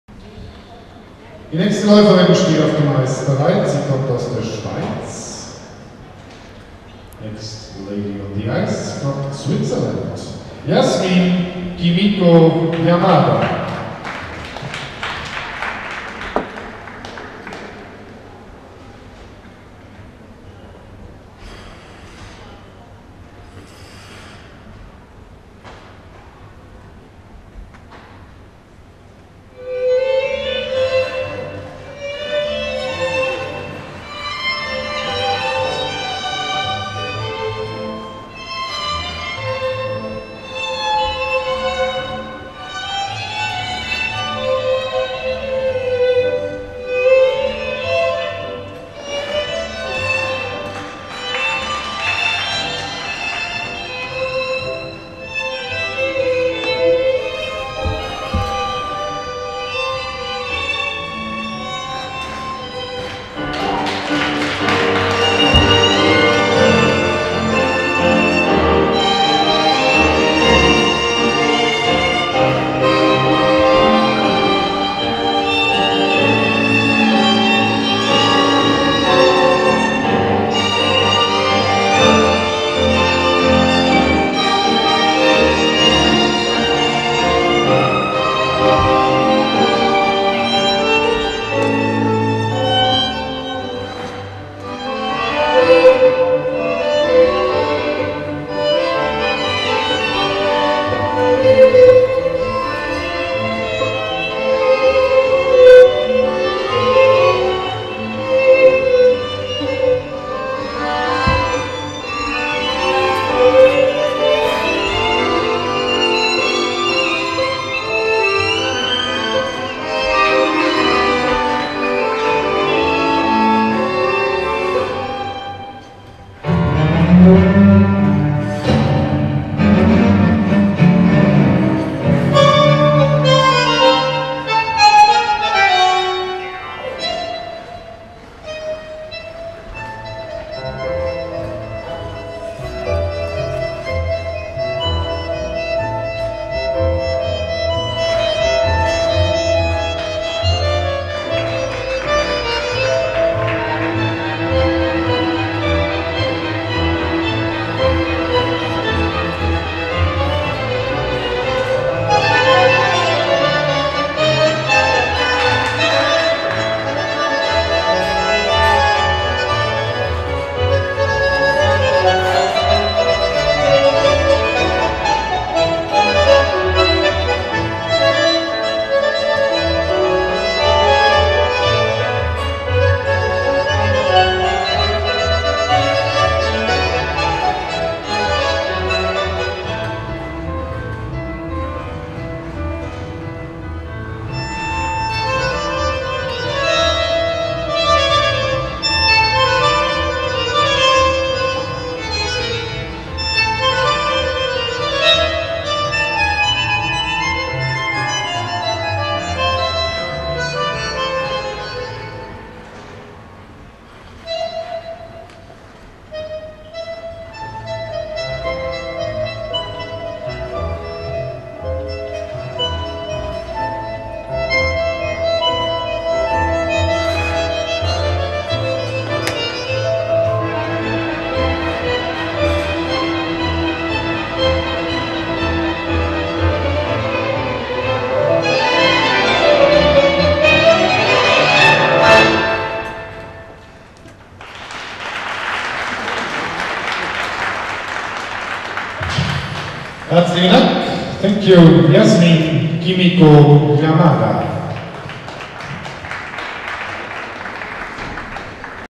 Yasmine Kimiko YAMADA SUI 119,22p
NRW Trophy for Figure & Pair Skating
JUNIOR Ladies - Free Skating